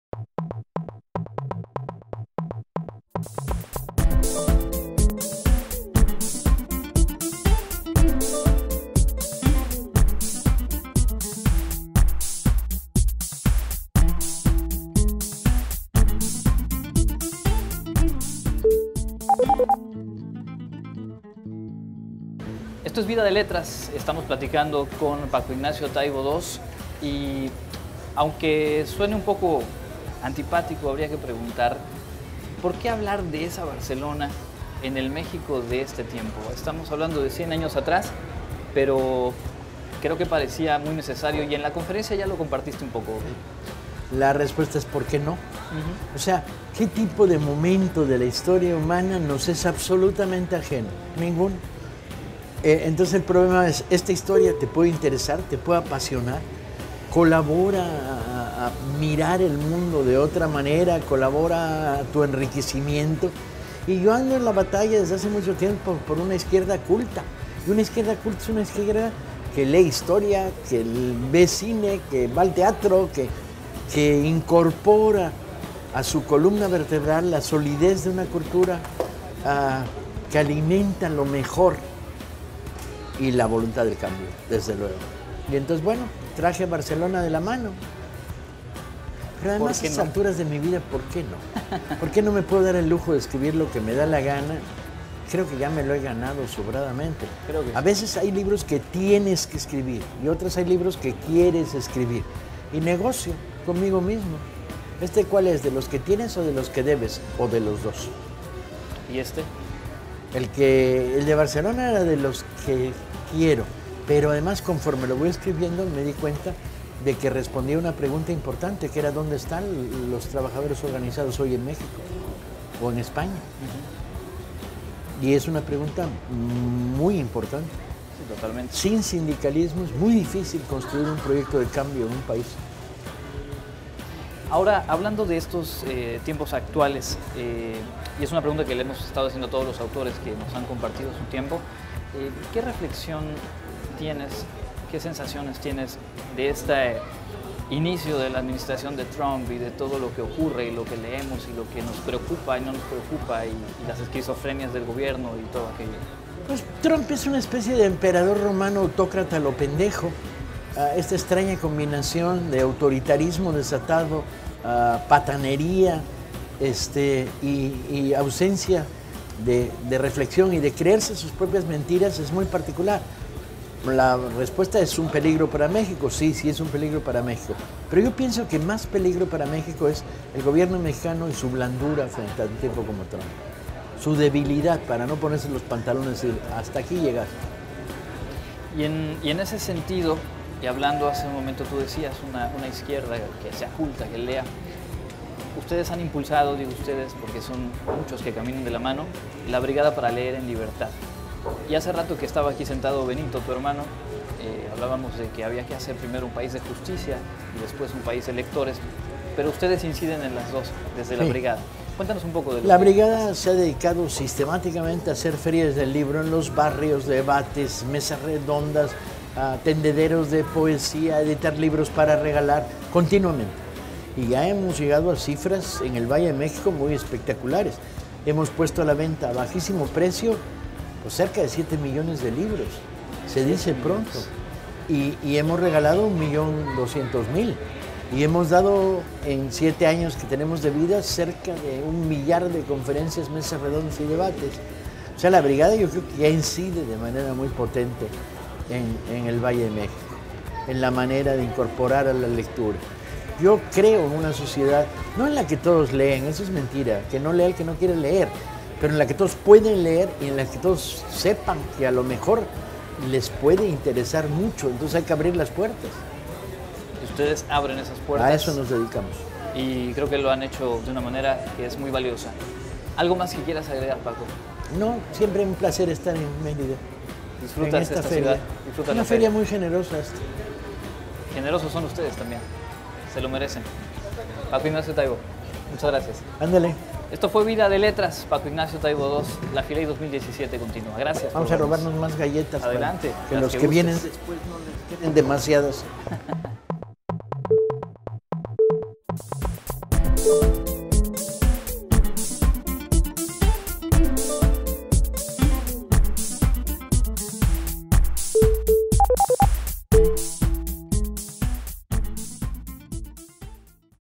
Parte 2.- Entrevista Paco Ignacio Taibo II 2
Entrevistamos al escritor Paco Ignacio Taibo II durante su asistencia a la FILEY 2017.